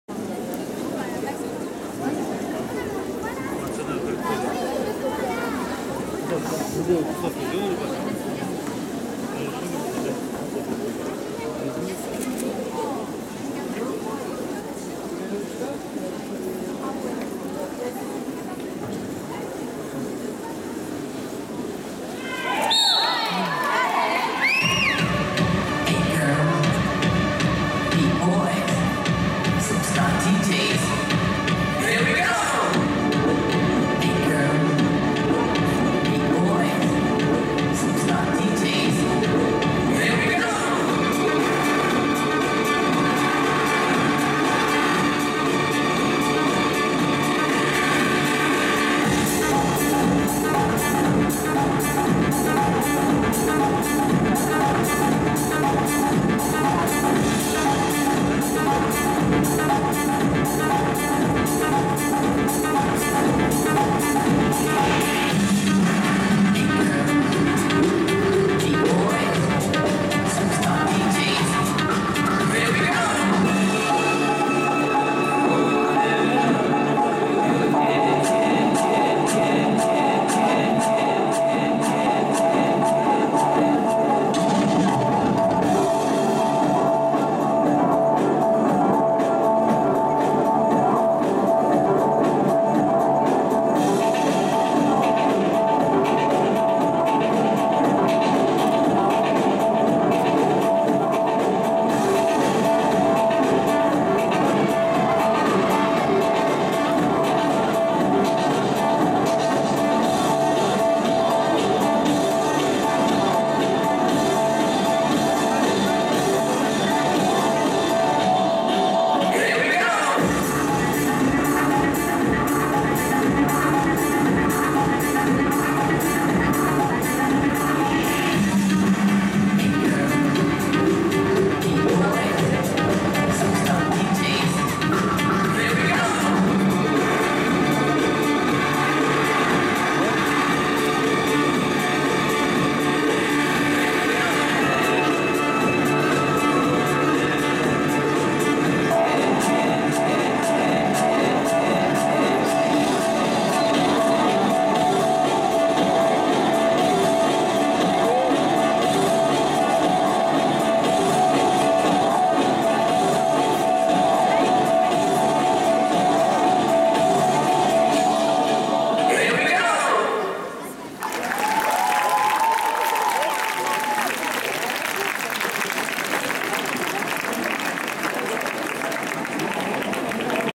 FRANCE Technical mixed duet - 2019 French Open Montreuil
Madeline PHILIPPE / Quentin RAKOTOMALALA

'19 French Open in Montreuil

71.8434  (Ex: 22.0000 / Impr.Art: 22.0000 / Diff: 27.8434)
Rank : 4th